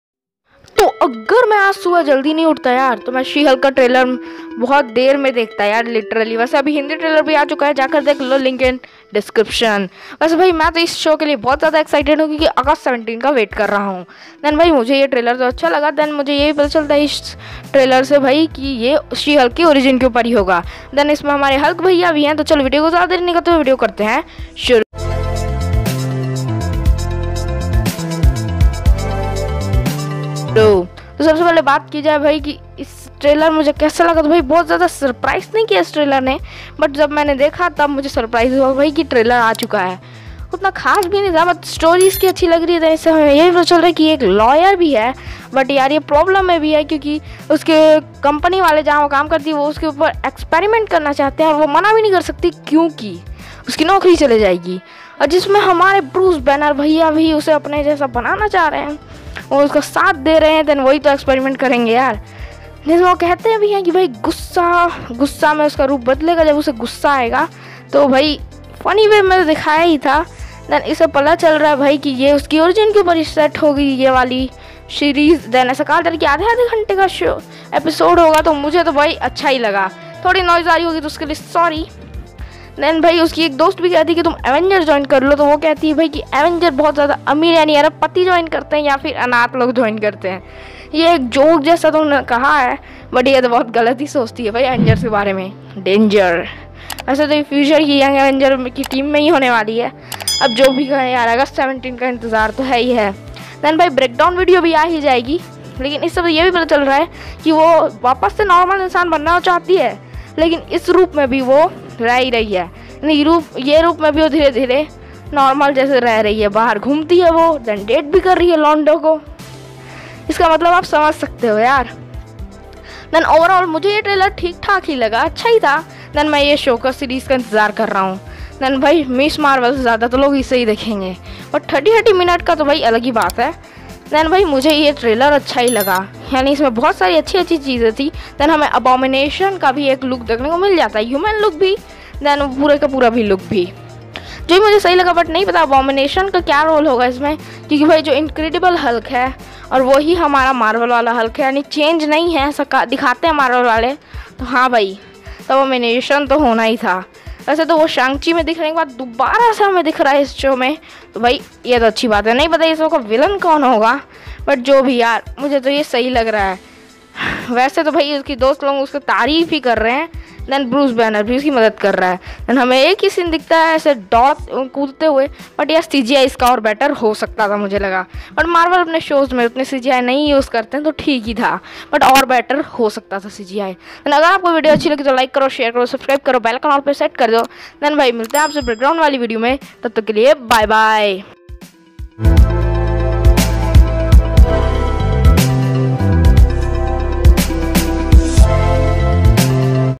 She Hulk Official Trailer Review In Hindi : CGI Aur Better Ho Sakta Tha - Amz Talks
She Hulk Trailer Review In Hindi : CGI Aur Better Ho Sakta Tha - Amz Talks

Video Credit - Marvel Studios